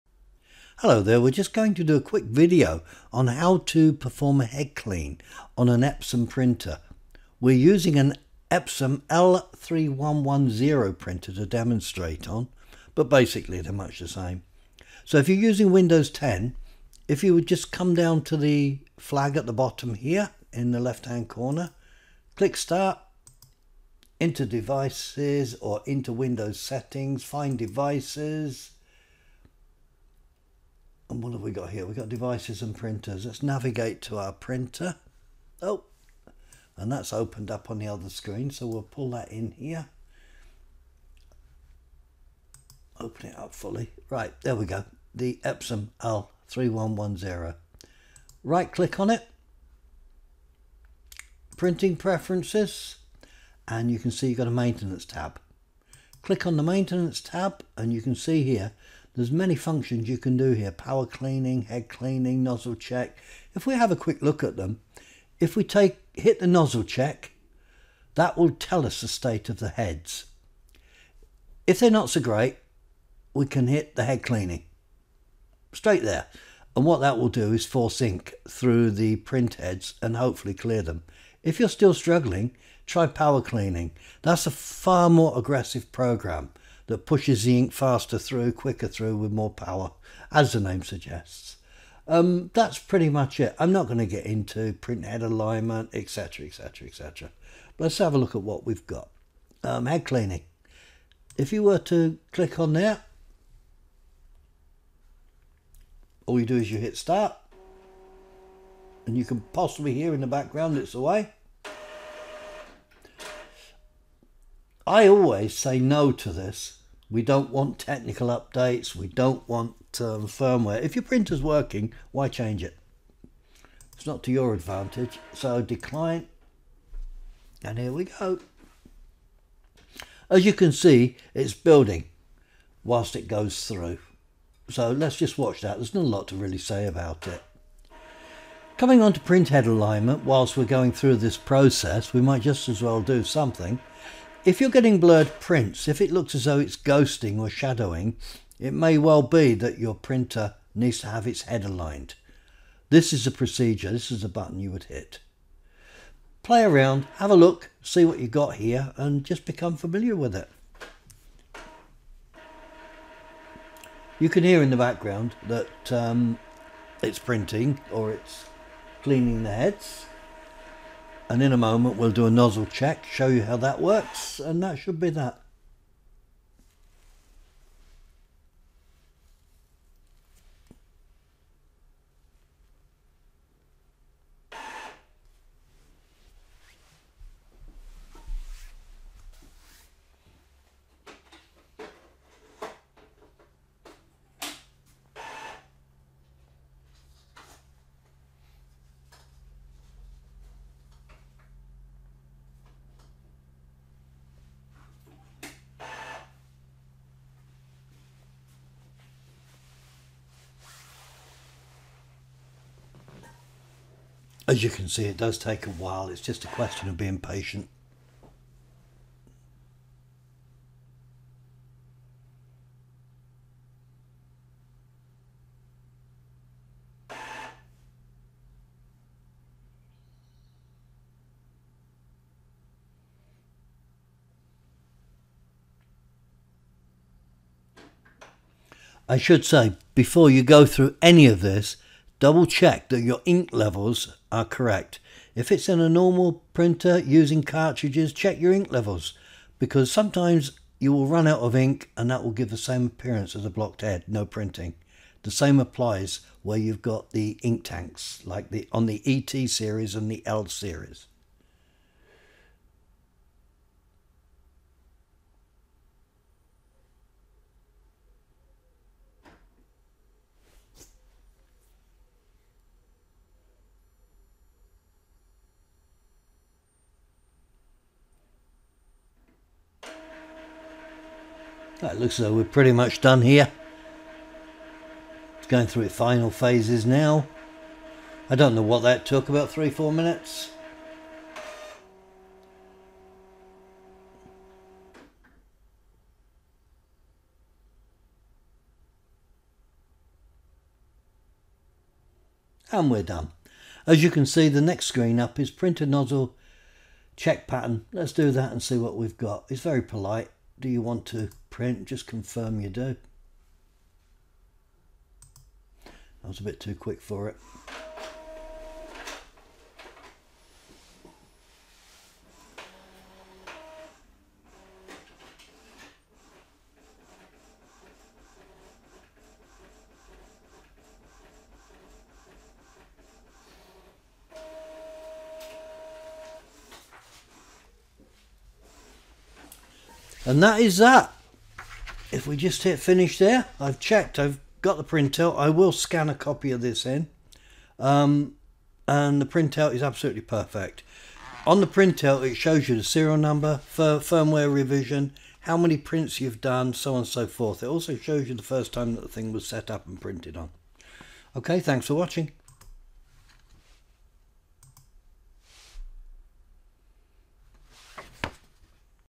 Head Cleaning an Epson L3110
A screen capture video of using the printer maintenance software to clean the Epson L3110 print heads.